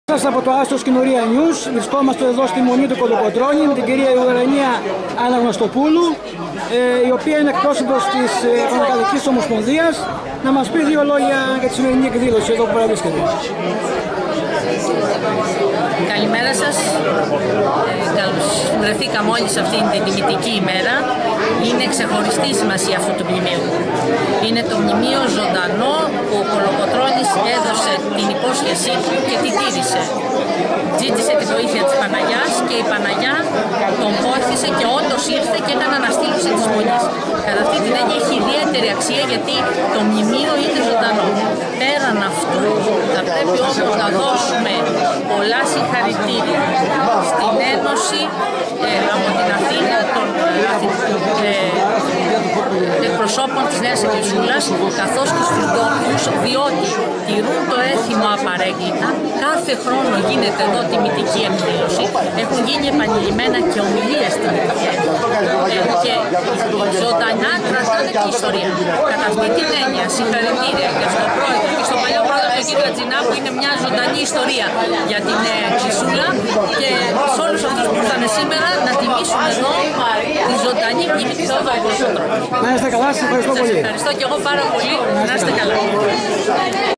astros-kynourianews.g:Μέλος Δ.Σ της Παναρκαδικής
ΤΟ Μέλος Δ.Σ της Παναρκαδικής, κ.Α.Αναγνωστοπούλου που παρευρέθηκε  στο "Αντάμωμα των Αντζιναίων"στο Μοναστήρι του Κολοκοτρώνη.

Ρεπορταζ:Φώτης Τζιβελόπουλος